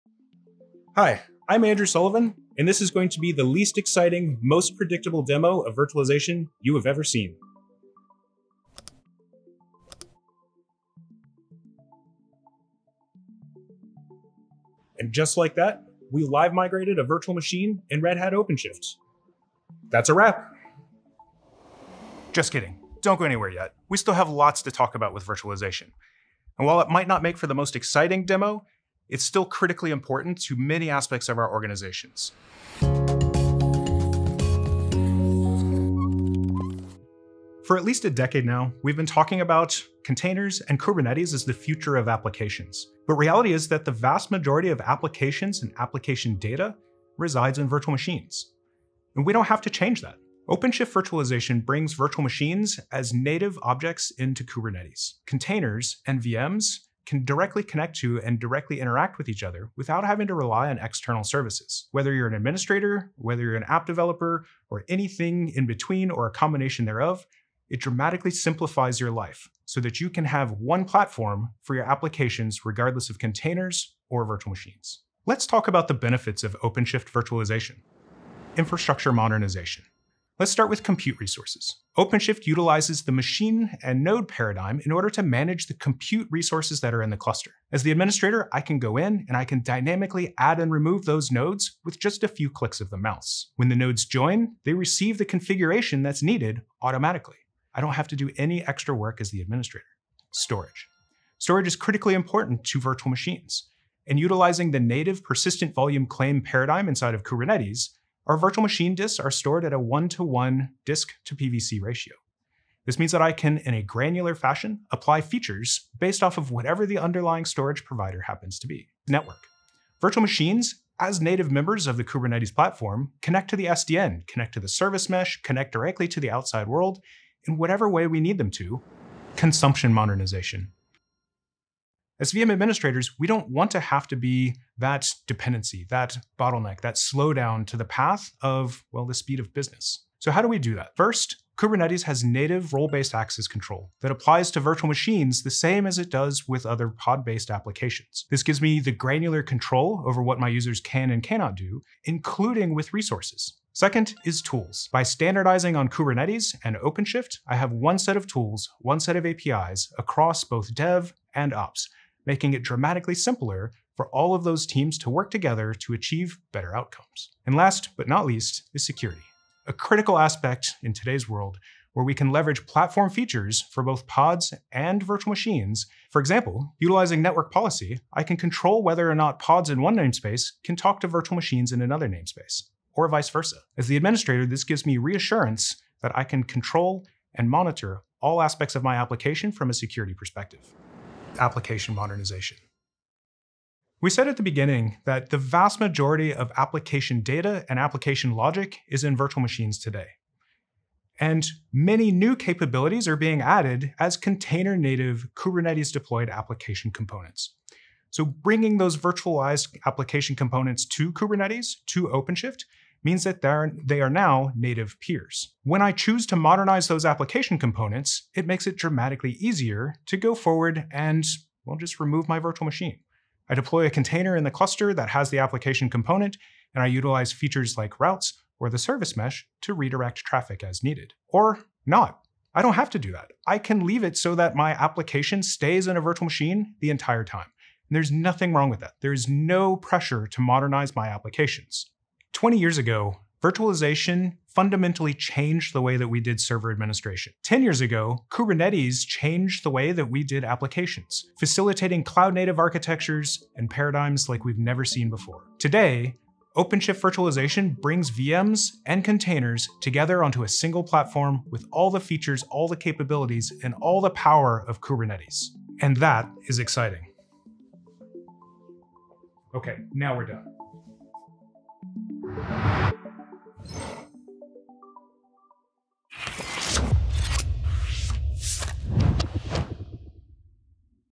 Modernize virtualization with Red Hat OpenShift
Is virtualization the most exciting technology out there? Probably not. But even if the innovation is happening in the container and cloud-native world, that doesn’t mean that virtual machines can’t benefit.

Join Andrew Sullivan, Senior Manager, Solution Patterns and Architecture, as he explores how Red Hat® OpenShift® Virtualization unites VMs and containers on a one comprehensive application platform, unlocking the benefits of modern approaches and tools for all your workloads.

With it, you can increase operational efficiency, provide flexibility for diverse workloads, and accelerate application development and deployment.

00:00 Intro
00:36 VMs and containers side-by-side on OpenShift
01:16 Compute resources, storage, and network
02:09 Access control, tooling, and security
03:10 Modernize virtual applications (or don’t)
04:02 Next steps